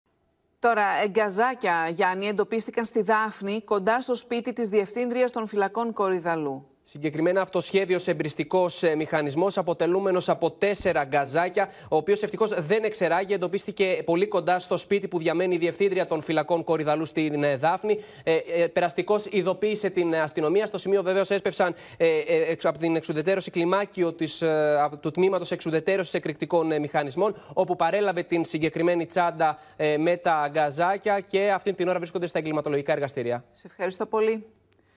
Συναγερμός στη Δάφνη: Έβγαλαν γκαζάκια κοντά στο σπίτι της διευθύντριας των φυλακών Κορυδαλλού
Γίνετε μέλος στο κανάλι του OPEN για να αποκτήσετε πρόσβαση σε προνόμια:
Γκαζάκια εντοπίστηκαν στη Δάφνη κοντά στο σπίτι της διευθυντρίας των φυλάκων Κορυδαλλού.